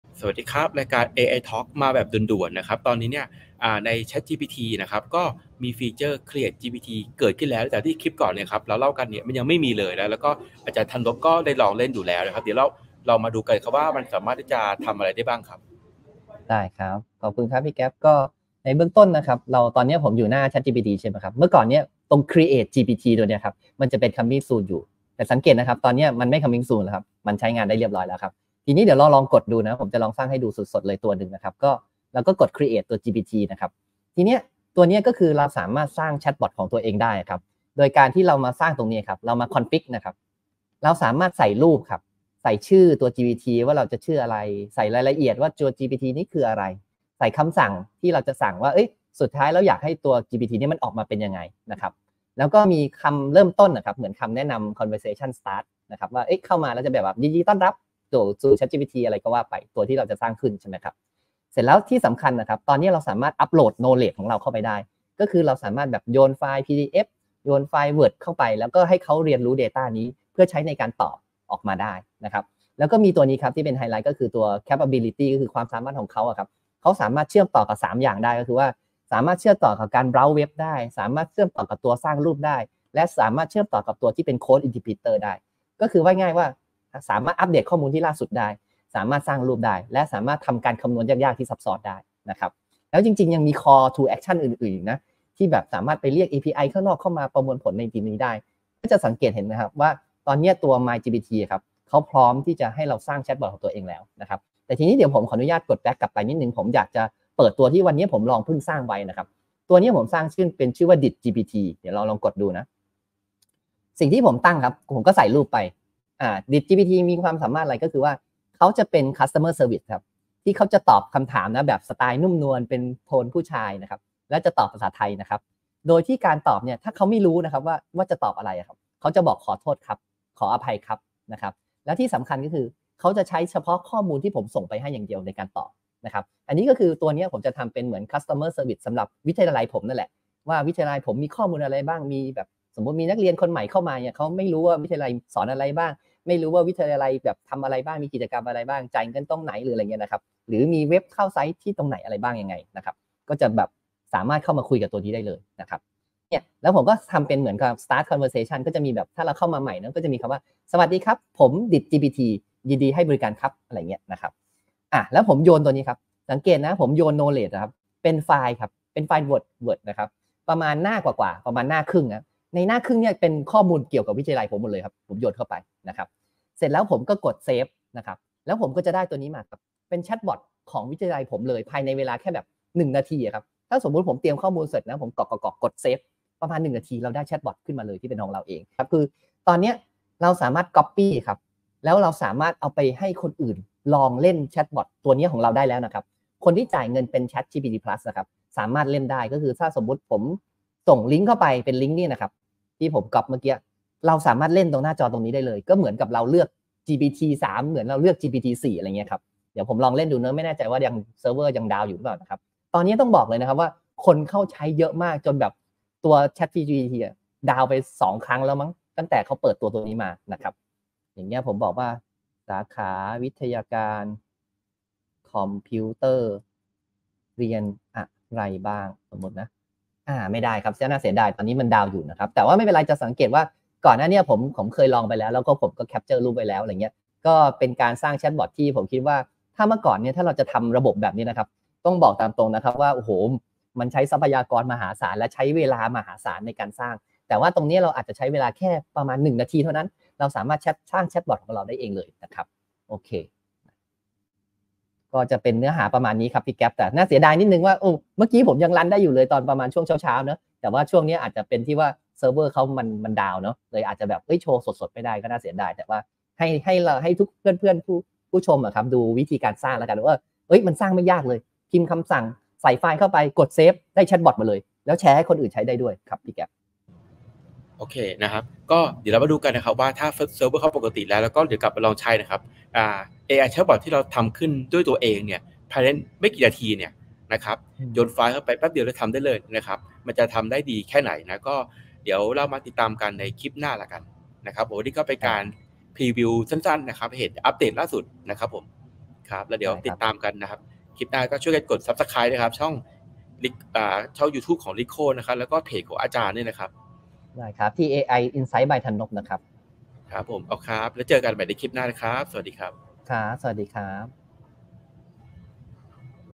พรีวิว Create a GPT สร้างเอไอแชทบอทด้วยตัวเองใน 1 นาที!!
หลังจาก OpenAI ได้ประกาศฟีเจอร์ Create a GPT ซึ่งเปิดให้สามารถสร้างเอไอแชทบอทของตัวเองได้อย่างรวดเร็ว ตอนนี้เริ่มเปิดให้ผู้ใช้บางรายทดลองใช้แล้วแบบ Beta ไปดูกันว่ามีขั้นตอนการทำอย่างไร